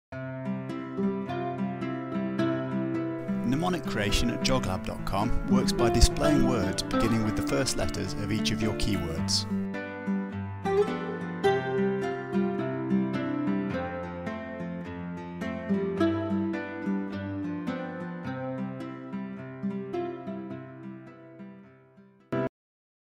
Mnemonic for Seven Deadly Sins
A mnemonic to fix in the mind Seven Deadly Sins could use the keywords: Lust, Gluttony, Greed, Sloth, Wrath, Envy, Pride.